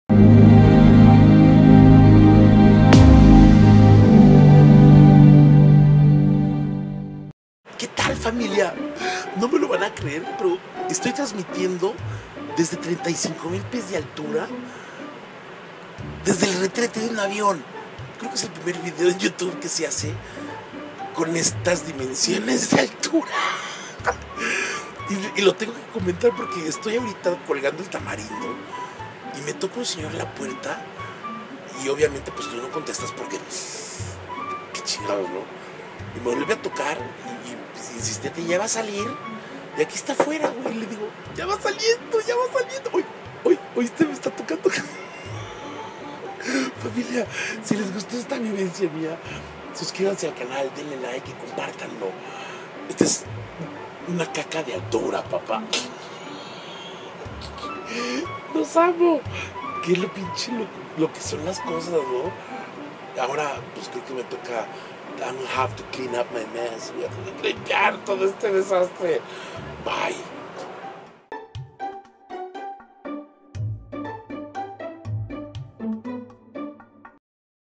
"EN EL BAÑO DE UN AVIÓN" by Lalo Manzano
Rie con lo que me paso a 35mil pies de altura!!!